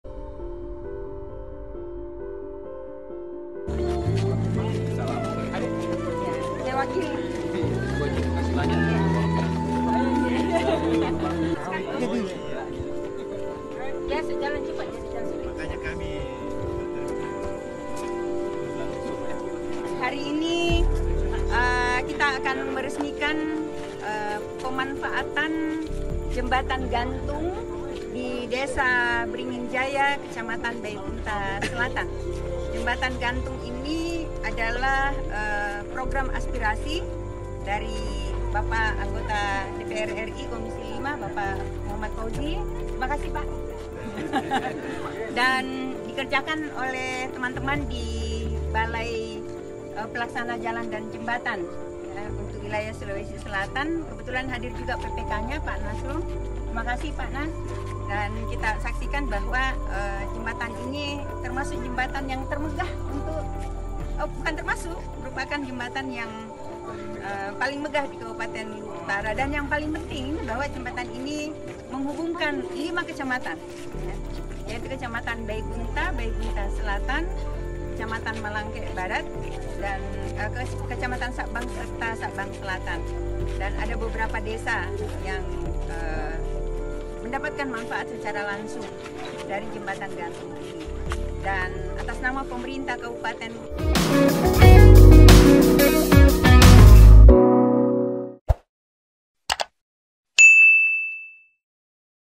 Muhammad Fauzi Resmikan Jembatan Beringin Jaya
Anggota DPR RI Dapil Sulsel III Muhammad Fauzi meresmikan Jembatan Beringin Jaya di Kecamatan Baebunta Selatan, Luwu Utara, pada Rabu 26 April 2023.

Jembatan gantung dengan panjang 120 meter ini menelan anggaran Rp4,3 miliar. Kehadiran jembatan tersebut tak lepas dari perjuangan dan aspirasi Anggota Komisi V DPR RI Muhammad Fauzi lewat Kementerian PUPR.

“Melalui program aspirasi di DPR RI, jembatan gantung Beringin Jaya ini hadir sebagai salah satu bentuk tanggung jawab saya kepada masyarakat di dapil Sulsel III khususnya di Kabupaten Luwu Utara,” kata Abang Fauzi, sapaan akrabnya, saat peresmian.

Video terkini dan terbaru..